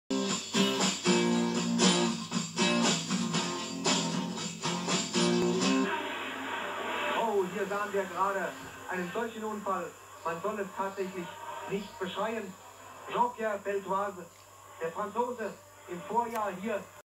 (REASSEMBLED VIDEO) JEAN PIERRE BELTOISE CRASH MONACO GP 1973
Il vincitore 1972 sull acqua  nn ebbe la  stessa abilità sul'asciutto l'anno dopo
Uscito largo al Casinò finì sul rail della curva del Mirabeau.